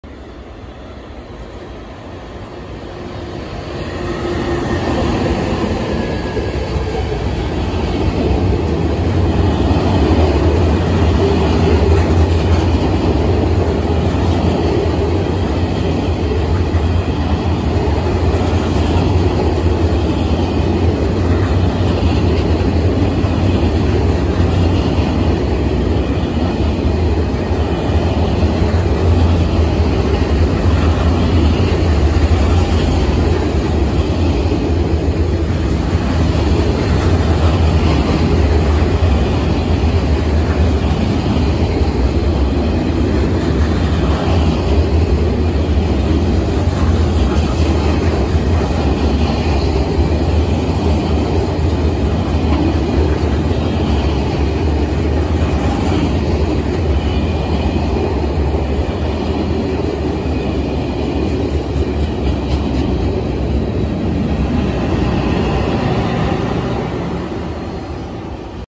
7825 Calais to Folkestone
Euroshuttle locos 9721 n 9707 working the 7825 Calais (France) to Folkestone (UK)